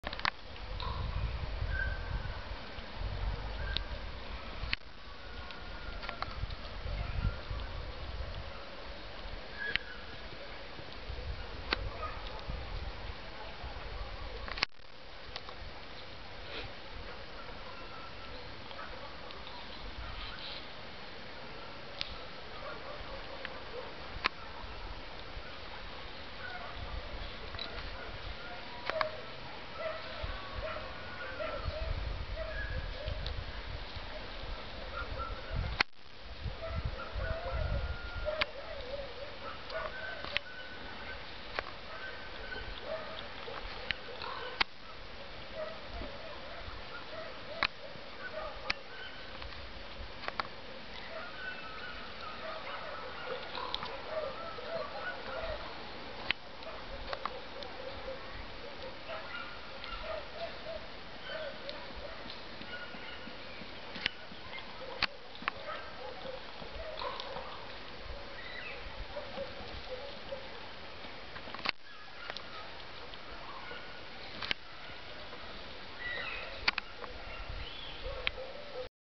Pretty squirell filmed very close- part 2.
Close to  a  pretty  squrell in  Covasna  forest, September 20, 2010 .